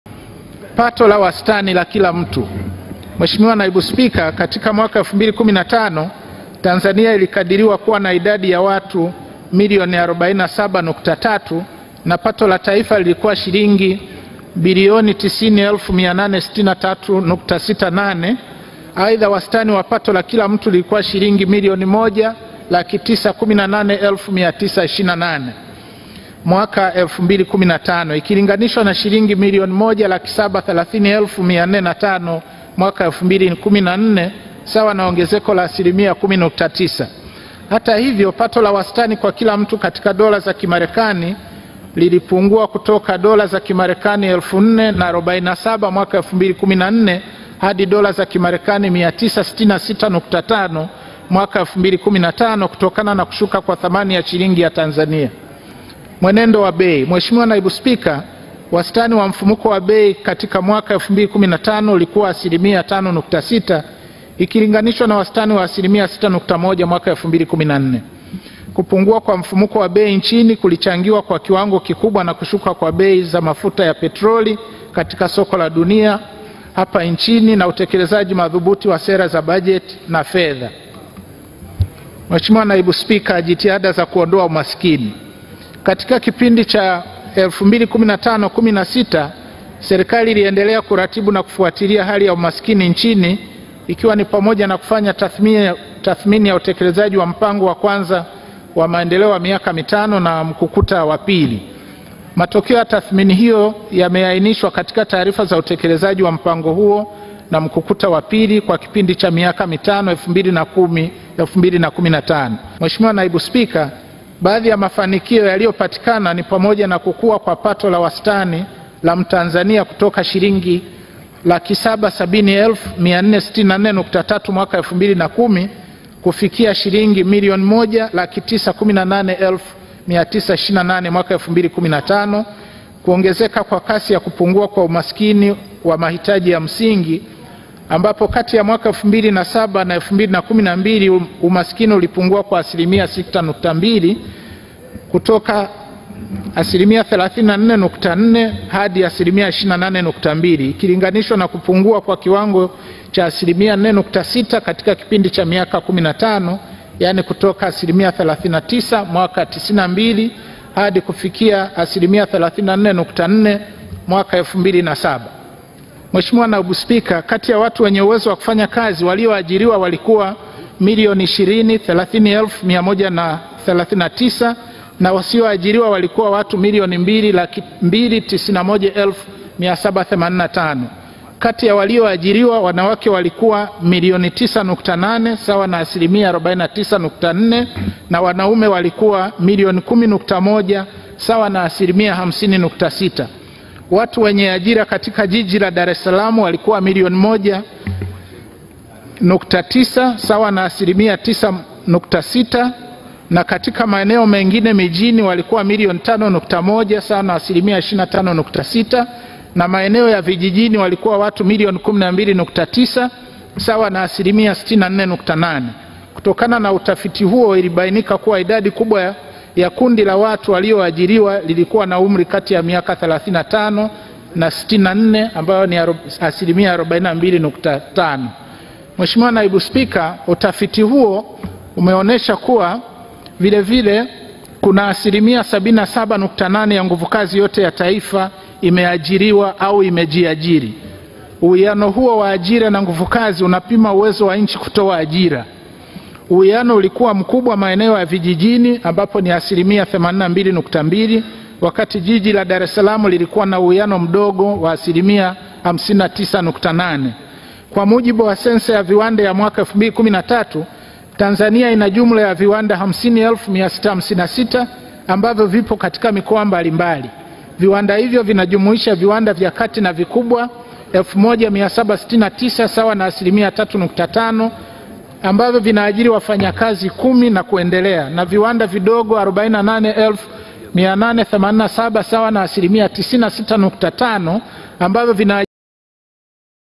Mapitio ya bajeti kuelekea bajeti kuu 2016/17
Waziri wa Fedha,  Dr. Philip Mpango ametoa tathmini ya bajeti iliyoisha kuelekea kusoma bajeti kuu ya Tanzania kwa mwaka 2016/17.